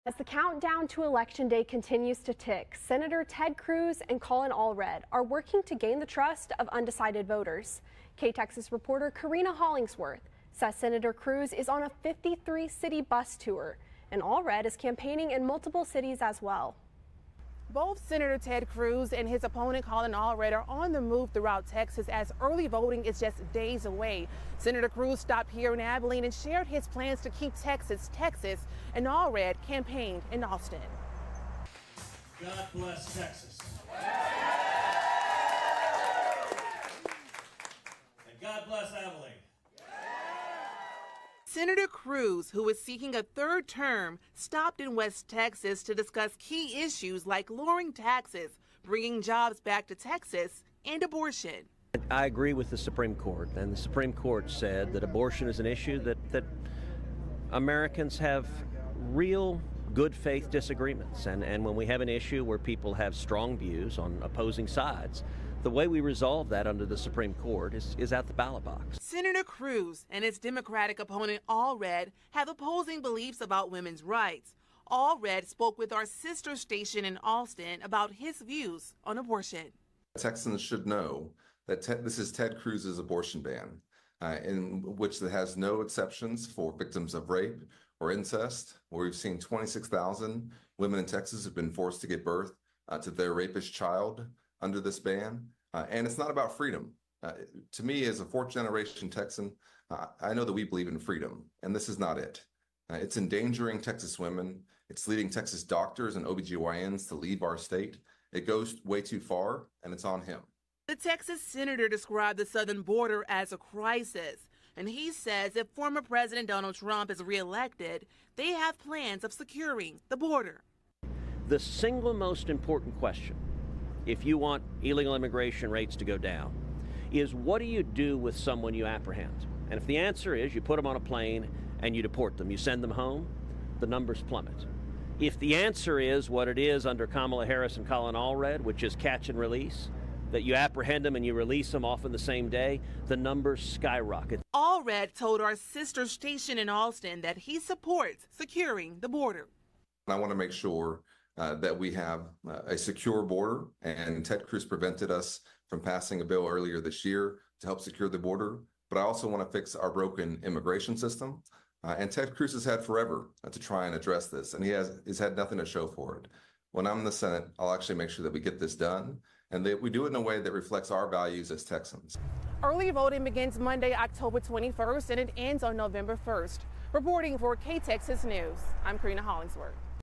Sen. Ted Cruz stops in Abilene on 53-city-bus tour
As the countdown to election day continues to tick, Senator Ted Cruz and his democratic opponent Collin Allred are on the move across Texas working to gain the trust of undecided voters.

Sen. Cruz stopped in Abilene Friday on his 53-city bus tour while Allred campaigned in Austin.